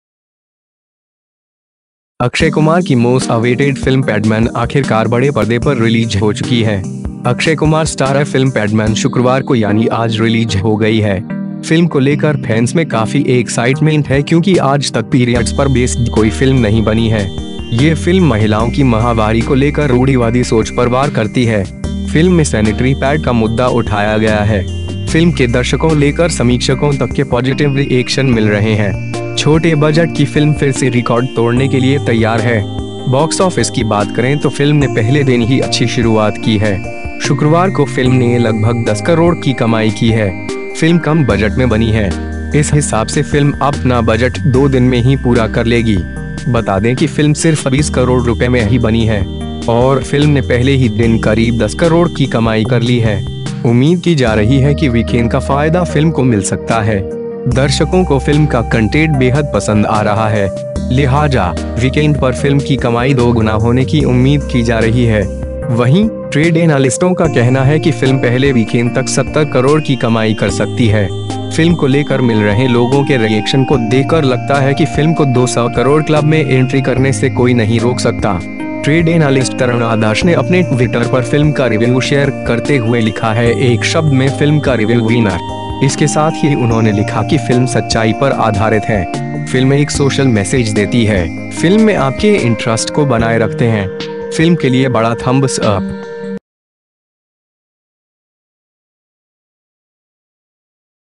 फिल्म 'पैडमैन' की धमाकेदार एंट्री, पहले दिन कमाए इतने करोड़ रुपये
अक्षय कुमार की मोस्ट अवेटेड फिल्म पैडमैन आखिरकार बड़े परदे पर रिलीज हो चुकी है। अक्षय कुमार स्टारर फिल्म 'पैडमैन' शुक्रवार को यानी आज रिलीज हो गई है। फिल्म को लेकर फैन्स में काफी एक्साइटमेंट है क्योंकि आज तक पीरियड्स पर बेस्ड कोई फिल्म नहीं बनी है। ये फिल्म महिलाओं की महावारी को लेकर रूढ़ीवादी सोच पर वार करती है। फिल्म में सैनिटरी पैड का मुद्दा उठाया गया है। फिल्म के दर्शकों लेकर समीक्षकों तक के पॉजिटिव रिएक्शन मिल रहे हैं। छोटे बजट की फिल्म फिर से रिकॉर्ड तोड़ने के लिए तैयार है। बॉक्स ऑफिस की बात करें तो फिल्म ने पहले दिन ही अच्छी शुरुआत की है। शुक्रवार को फिल्म ने लगभग 10 करोड़ की कमाई की है। फिल्म कम बजट में बनी हैं। इस हिसाब से फिल्म अपना बजट दो दिन में ही पूरा कर लेगी।

बता दें कि फिल्म सिर्फ 20 करोड़ रुपये में ही बनी है। और फिल्म ने पहले ही दिन करीब 10 करोड़ की कमाई कर ली है। उम्मीद की जा रही है कि वीकेंड का फायदा फिल्म को मिल सकता है।

दर्शकों को फिल्म का कंटेट बेहद पसंद आ रहा है। लिहाजा, वीकेंड पर फिल्म की कमाई दोगुना होने की उम्मीद की जा रही है। वहीं, ट्रेड एनालिस्टों का कहना है कि फिल्म पहले वीकेंड तक 70 करोड़ की कमाई कर सकती है। फिल्म को लेकर मिल रहे लोगों के रिएक्शन को देखकर लगता है कि फिल्म को 200 करोड़ क्लब में एंट्री करने से कोई नहीं रोक सकता। ट्रेड एनालिस्ट तरण आदर्श ने अपने ट्विटर पर फिल्म का रिव्यू शेयर करते हुए लिखा है 'एक शब्द में फिल्म का रिव्यू वीनर। इसके साथ ही उन्होंने लिखा कि फिल्म सच्चाई पर आधारित है। फिल्म एक सोशल मैसेज देती है, फिल्म में आपके इंट्रस्ट को बनाए रखते हैं। फिल्म के लिए बड़ा थंब्स अप'।